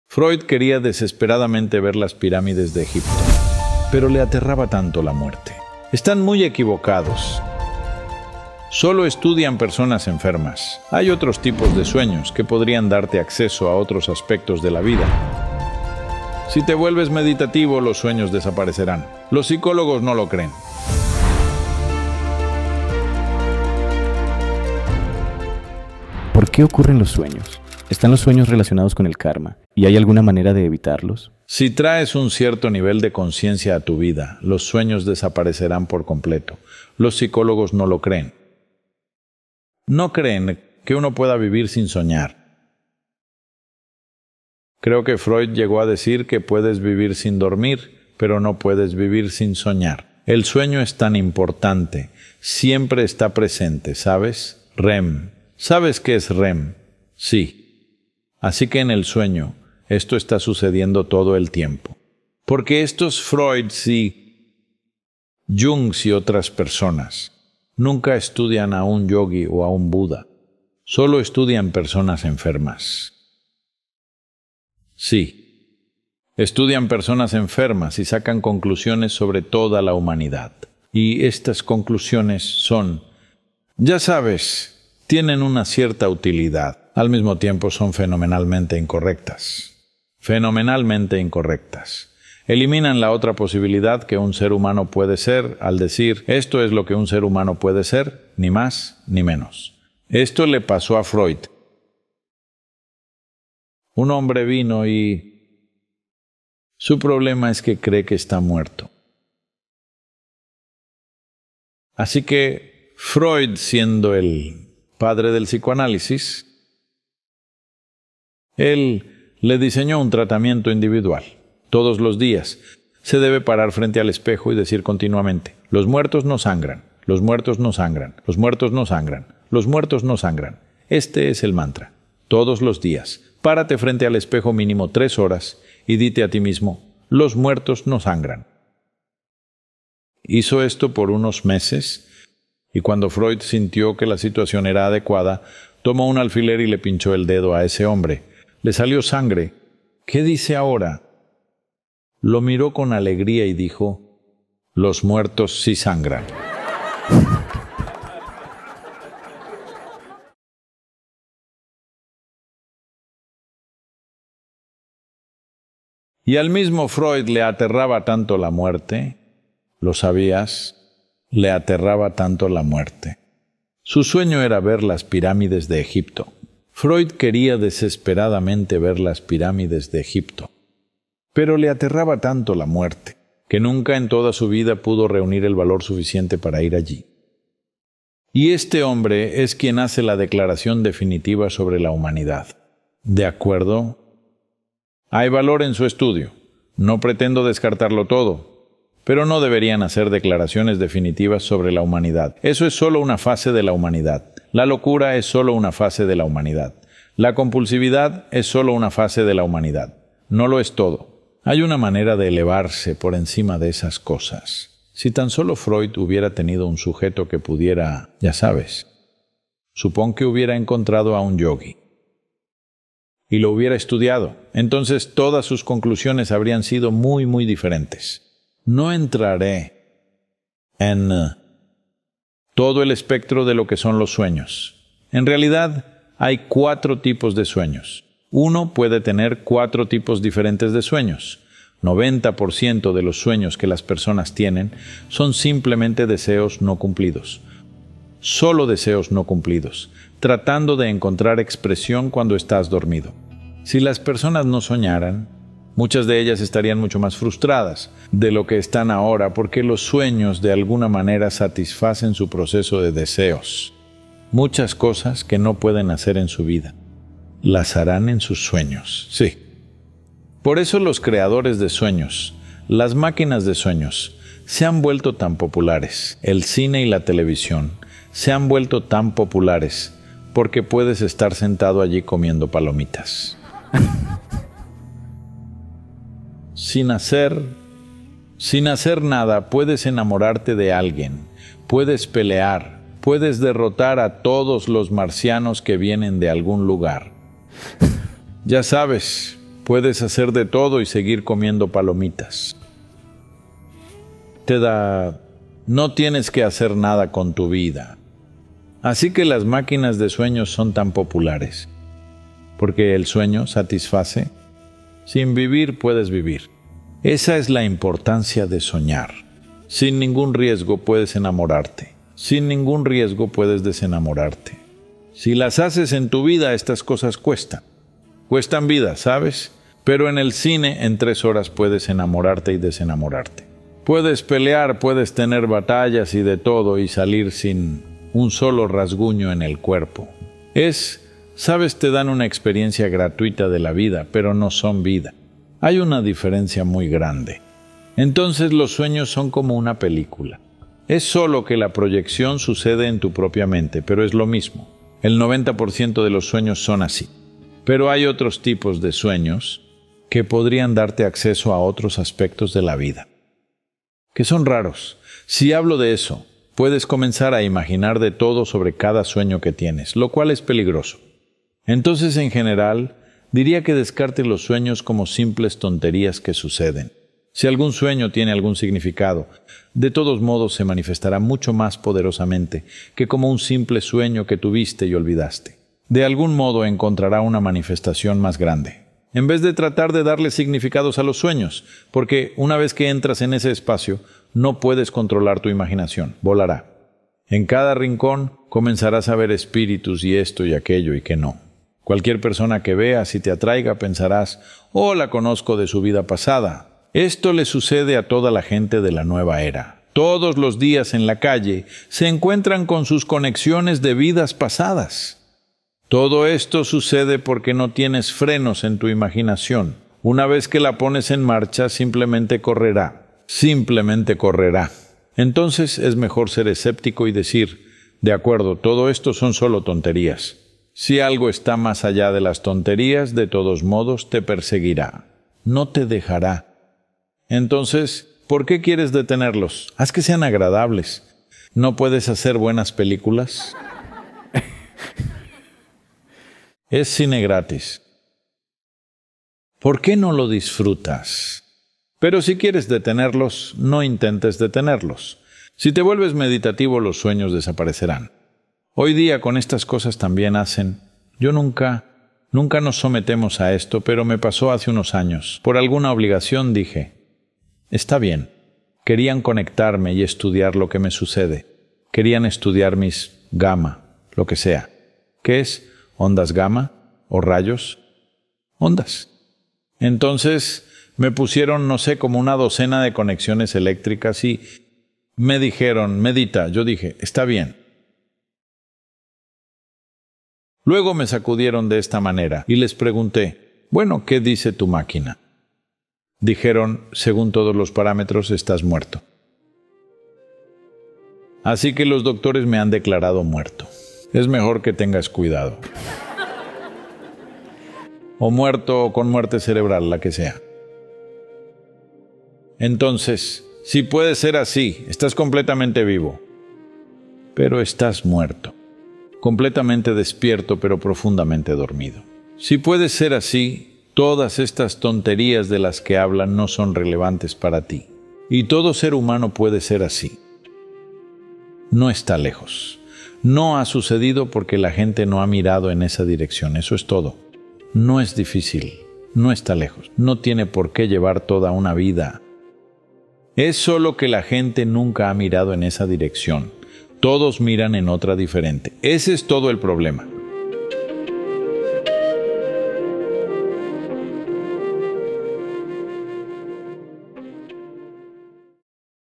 Lo que Freud no entendía sobre la mente | Sadhguru Español, voz doblada con IA
Sadhguru responde a una pregunta sobre el significado de los sueños y analiza cómo este aspecto de nuestra vida ha sido muy malinterpretado por los psicoanalistas.

La voz de Sadhguru en este video está doblada por un clon de IA.